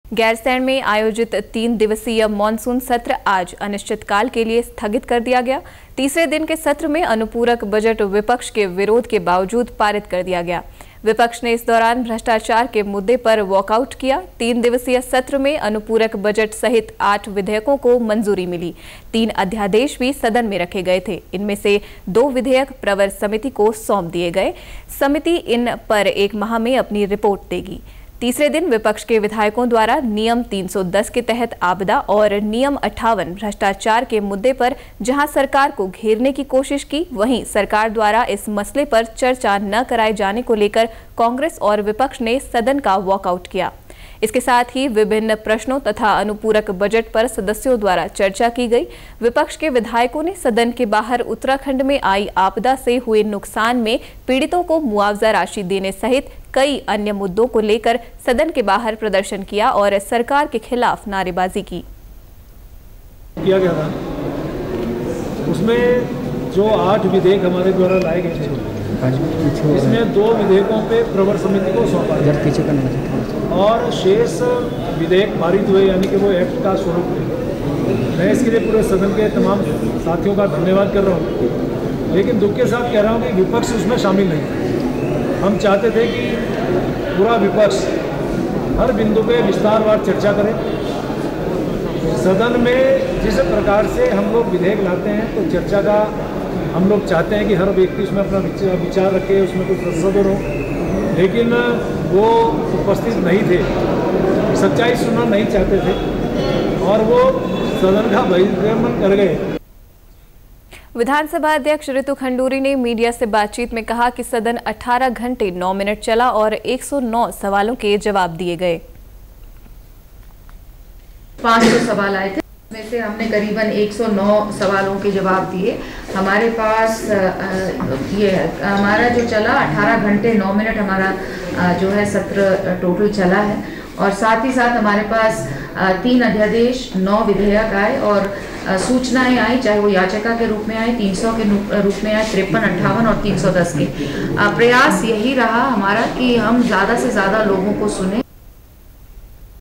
गैरसैंण में आयोजित तीन दिवसीय मानसून सत्र आज अनिश्चितकाल के लिए स्थगित
गैरसैंण में आयोजित तीन दिवसीय मानसून सत्र आज अनिश्चितकाल के लिए स्थगित कर दिया गया। तीसरे दिन के सत्र में अनुपूरक बजट विपक्ष के विरोध के बावजूद पारित कर दिया गया। विपक्ष ने इस दौरान भ्रष्टाचार के मुद्दे पर वॉकआउट किया।